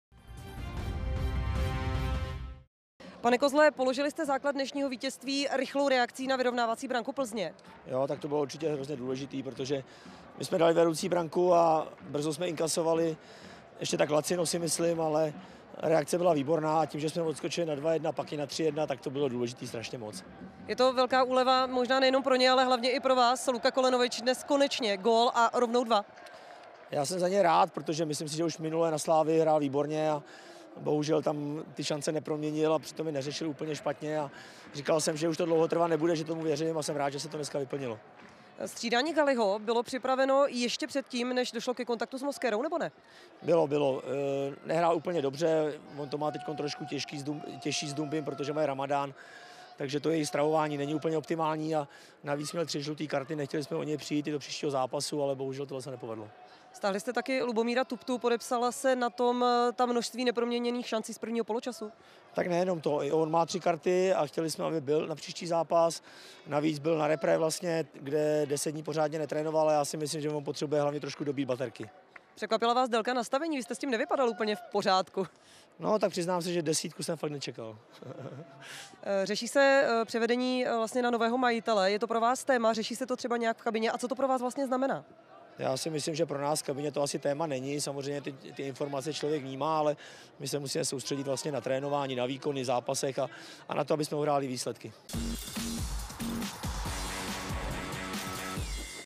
Liberec dokázal porazit Plzeň! Jak hodnotí zápas trenér Kozel?
Liberec zvládl i druhý zápas sezony s Plzní, v Doosan Aréně vyhrál 3:1. „Dali jsme vedoucí branku, lacino jsme pak brzy inkasovali, ale reakce byla výborná. To, že jsme odskočili nejprve na 2:1 a pak na 3:1, tak to bylo strašně moc důležité,“ uvedl trenér Kozel. Jak hodnotí výkon Kulenoviče?